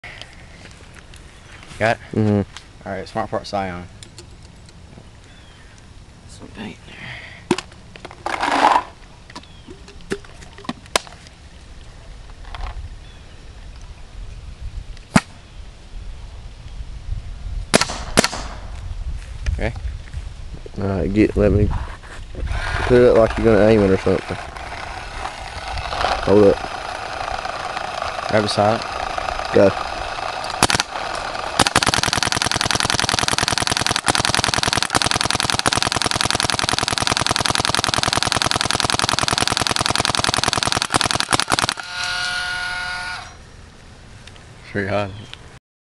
Smart Parts Ion Custom shooting ropes Part 1
yes it isn't as fast as i could have shot but i had it on Ramping low rate of fire next video will be 40 rate of fire!